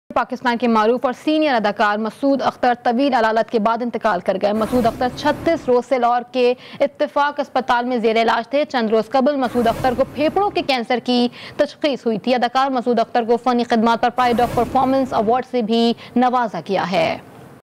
Well known and senior actor Masood Akhtar passed away after a long illness | Breaking News
Well known and senior actor Masood Akhtar passed away after a long illness | Breaking News | GTV Network HD